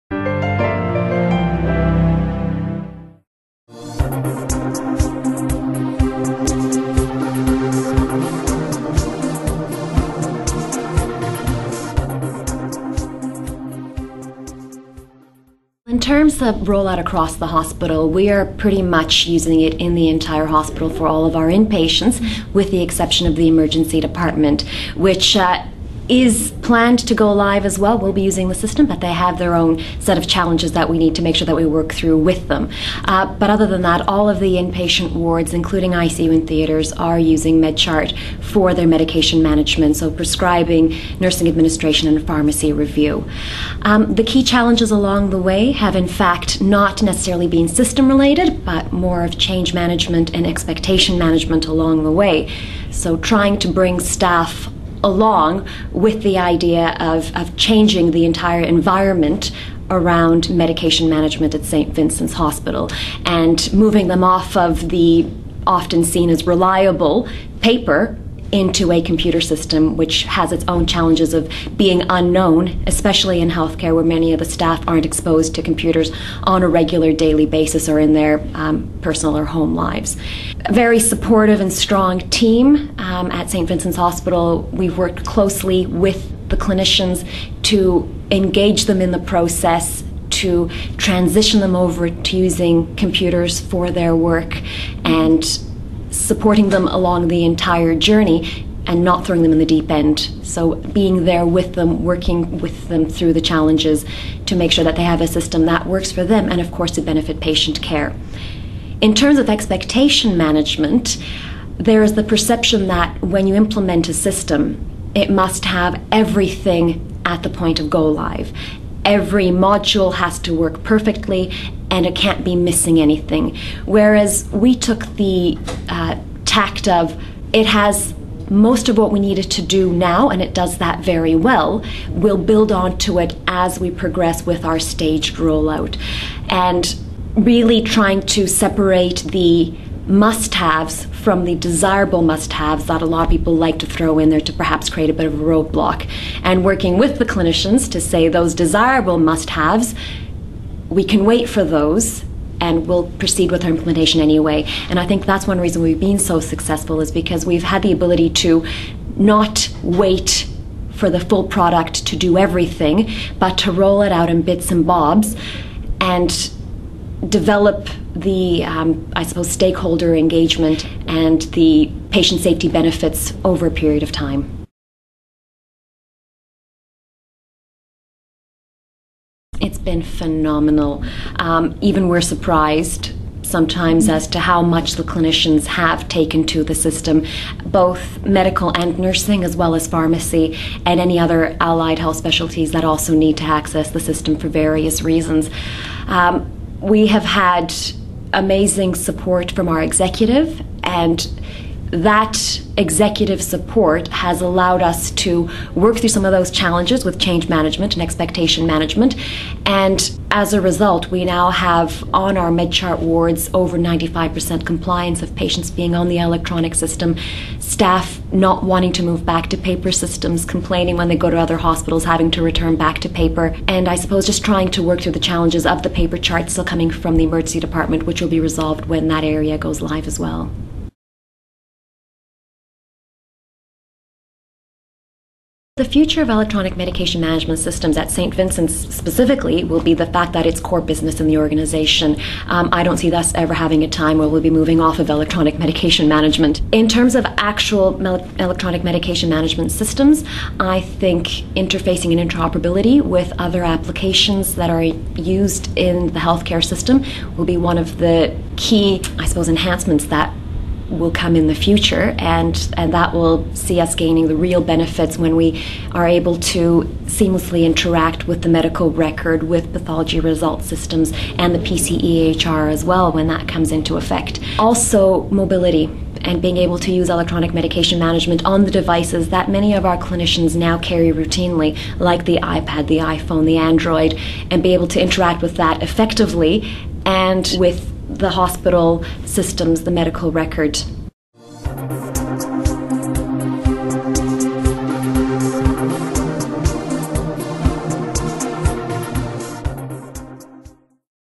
Electronic Medication Management 2012: Pre-event interview with Silvia Fazekas
Joining me today is Silvia Fazekas, Clinical Information Systems Manager at St Vincent's Public Hospital. Silvia will be speaking at IIR's Electronic Medication Management conference on 21-22 June 2012 in Melbourne.

Questions:

1. St Vincent's have been using electronic medication management systems for some time now. Where are you in terms of full roll out across the hospital and what have been some of the key challenges faced?

2. What has the clinician uptake of electronic medication management systems been like at St Vincent's?

3. Where do you see the future of electronic medication management systems?